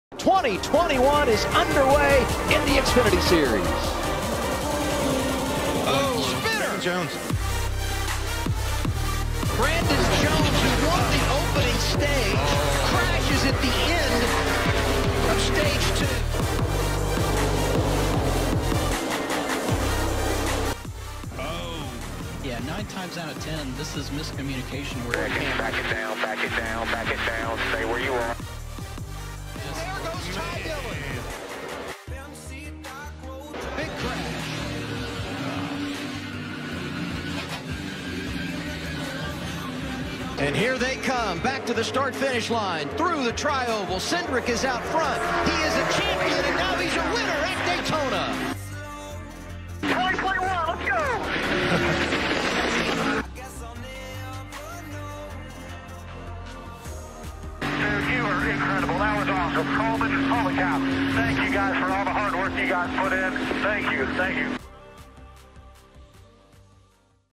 Nascar 2021 Xfinity series edit
An edit of the Xfinity race at Daytona International Speedway.